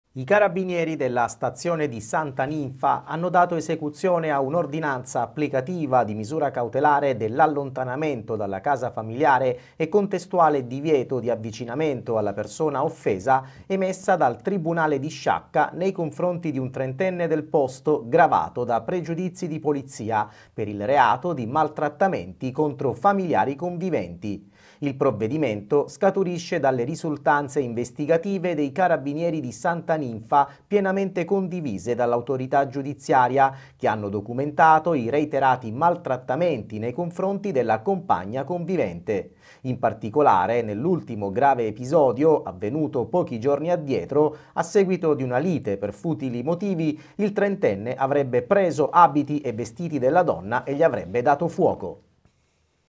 Prende i vestiti della convivente e gli dà fuoco: misura cautelare per un 30enne di Santa Ninfa
Prende i vestiti della convivente e gli dà fuoco: misura cautelare per un 30enne di Santa Ninfa, nel trapanese